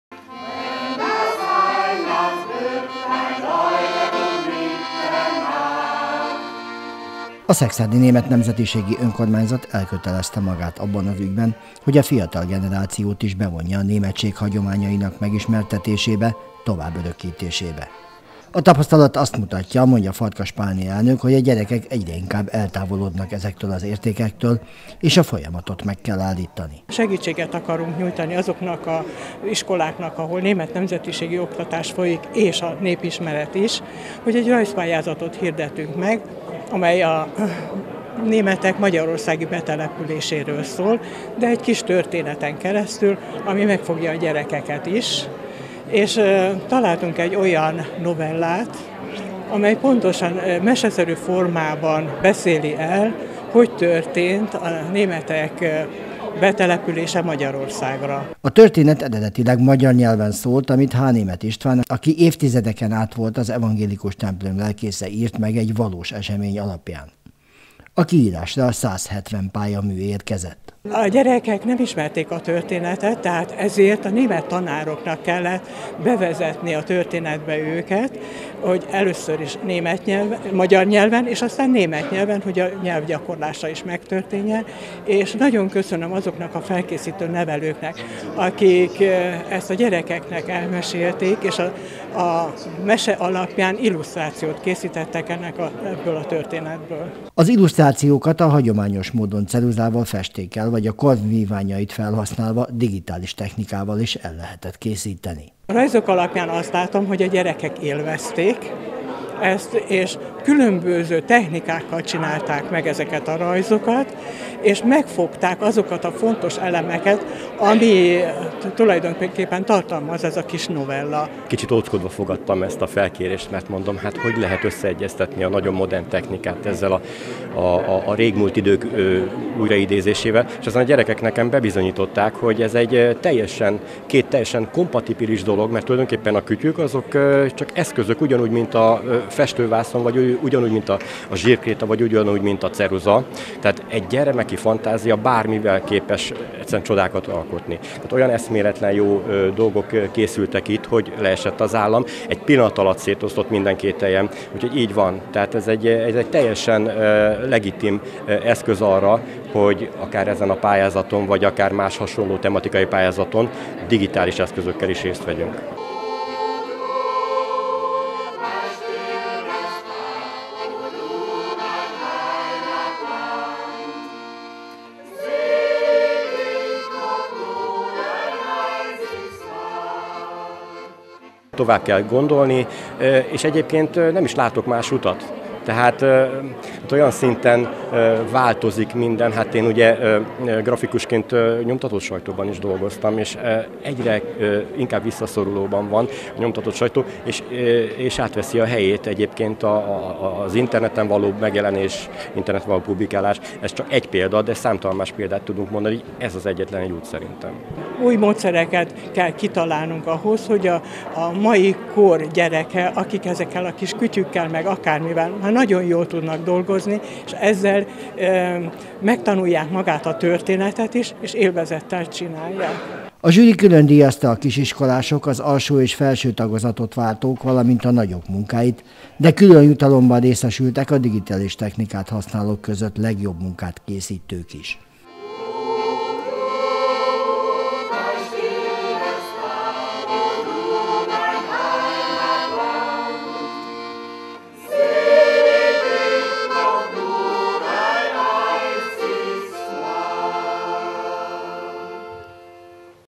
Rajzversenyt hirdettek Szekszárdon a németek betelepítésének történetéről
A digitális technika mind nagyobb szerephez jut a hagyományok őrzésében. Ez lehet a konklúziója a Szekszárdi Német Önkormányzat által kiírt rajzversenynek, amelyre számos, a modern kor vívmányával készült alkotással neveztek. A feladat az volt, hogy a gyerekek dolgozzák fel a németség betelepítéséről szóló történet. A kiírok több kategóriában is díjazták a legjobb pályamunkákat.

Die digitale Technik gewinnt eine immer größere Rolle in der Bewahrung der Traditionen. Das kann die Schlussfolgerung des von der Seksarder Deutschen Selbstverwaltung ausgeschriebenen Zeichenwettbewerbs sein, auf den zahlreiche Werke eingeschickt wurden, die mit den Errungenschaften der modernen Zeit angefertigt wurden. Die Aufgabe
war, dass die Kinder die Geschichte über die Ansiedlung der Deutschen bearbeiten. Die besten Arbeiten wurden in mehreren Kategorien ausgezeichnet.